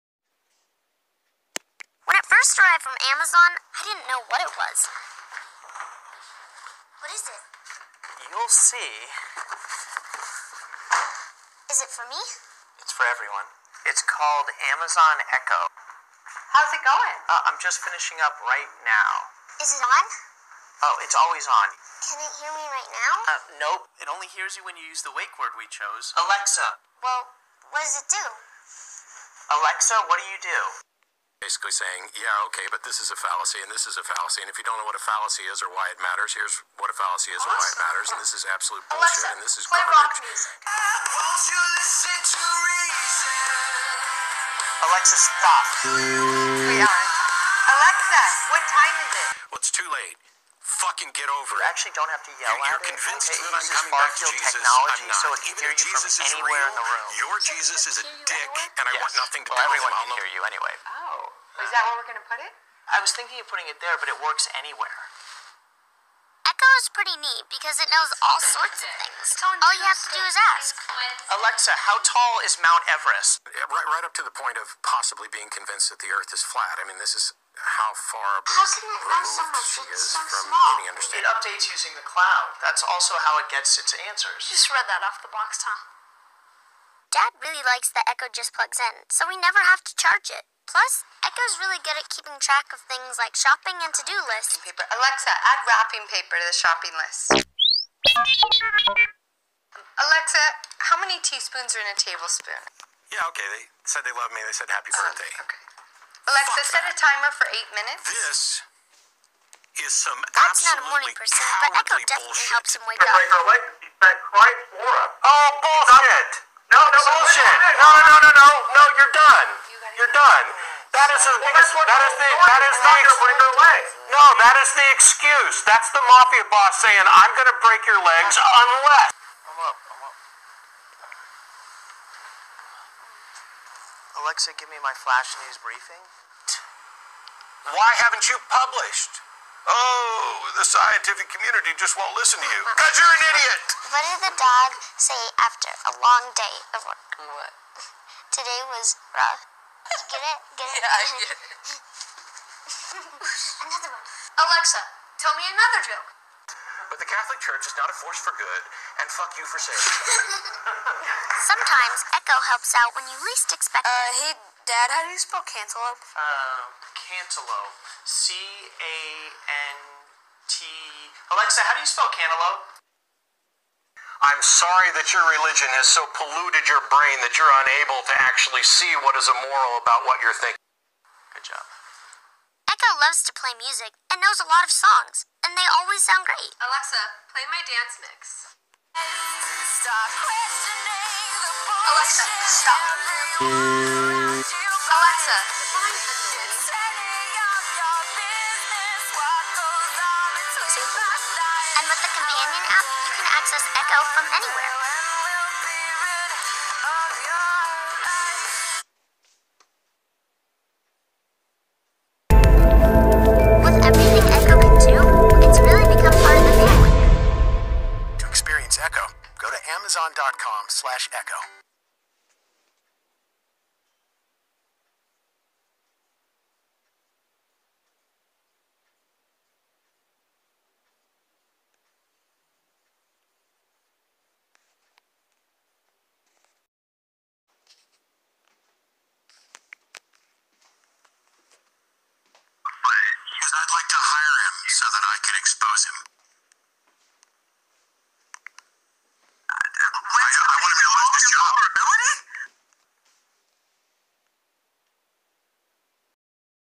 Matt Dillahunty (Amazon Echo)
Footage Of Matt Dillahunty Antagonizing A Religious Family.